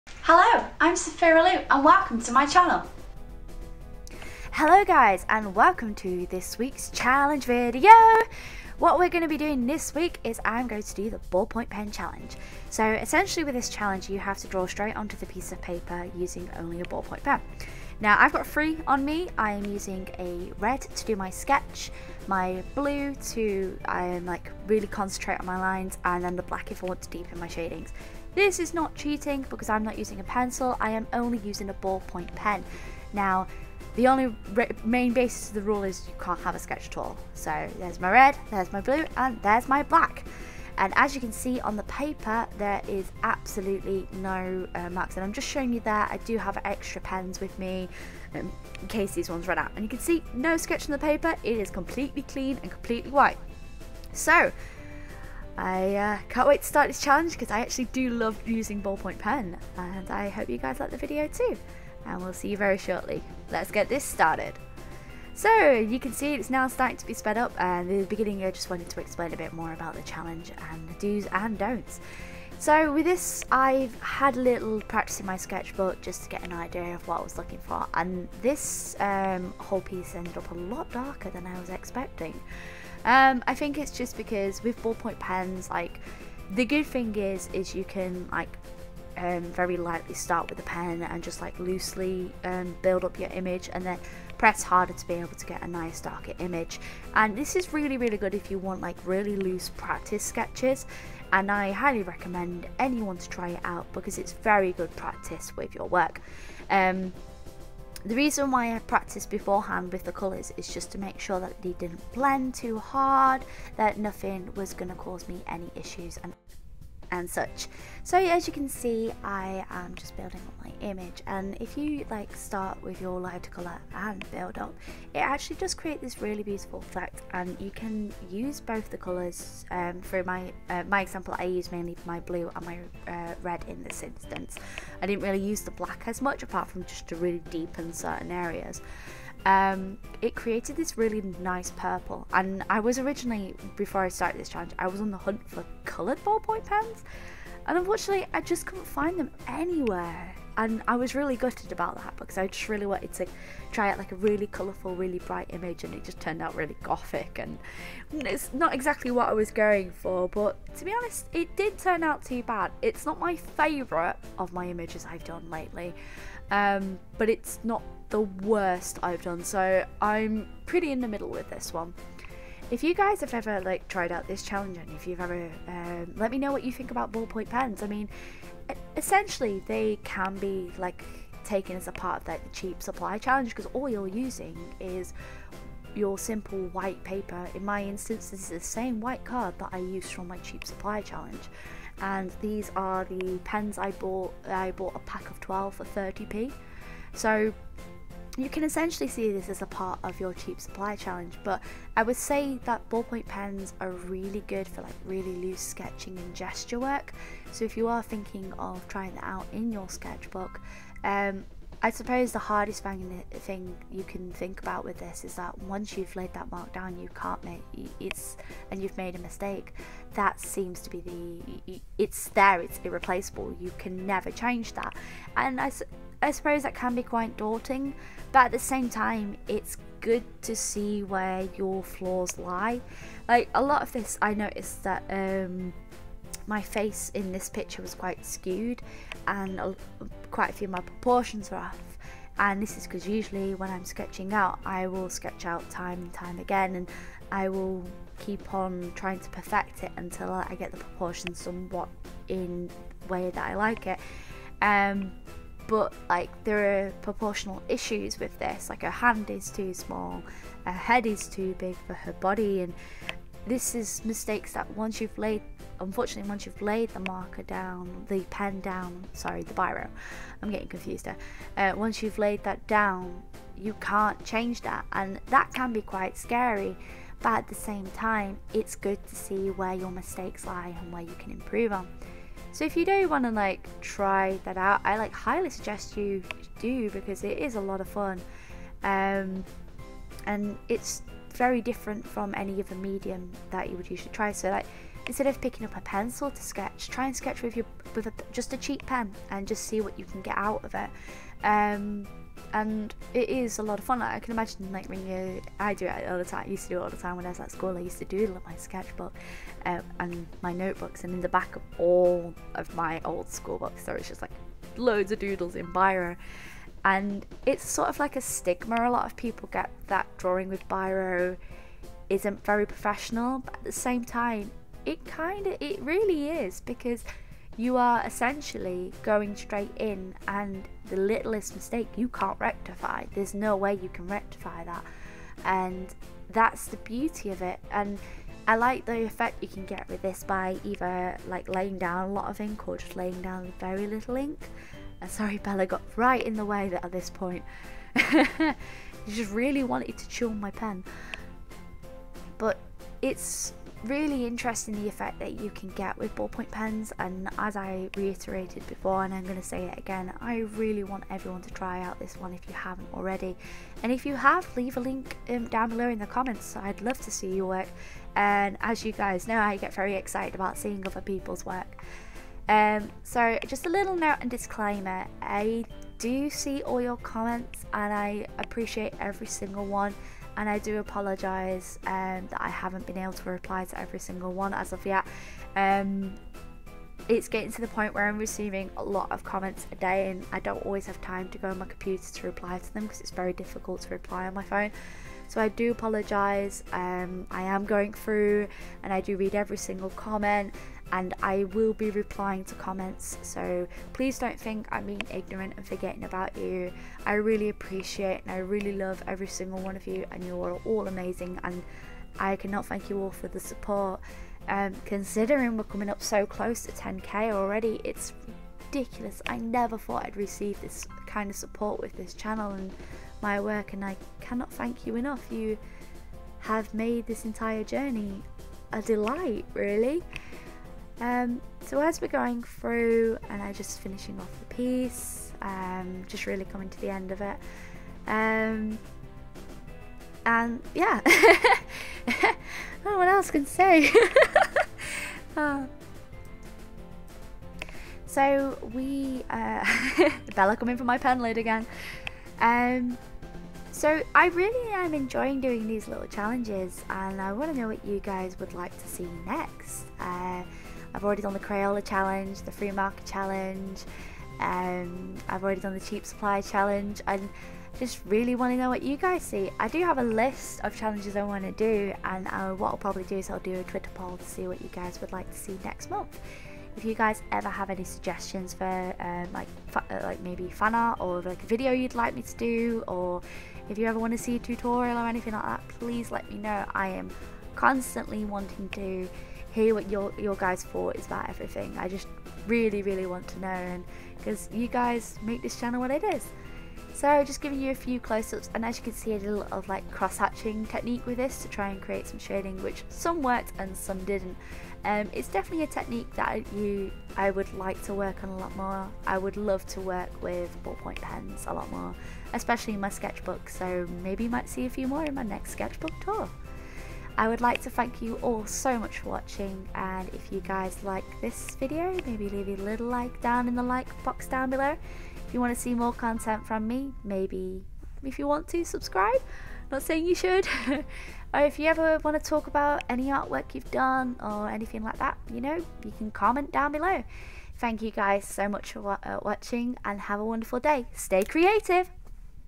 Ballpoint Pen Challenge
Hello guys this week is  ballpoint pen challenge week I do hope you enjoy this video x
Any suggestions on any arty challenges you would like me to try out, or if you have awesome artists or work for me to check out let me know in the comments .
Thank you all so much for watching and have a wonderful day.
Stay creative folks.

Places you can get letraset promarkers and other arty supplies from (please note i am in no way affiliated with these places i just get most of my supplies from them)-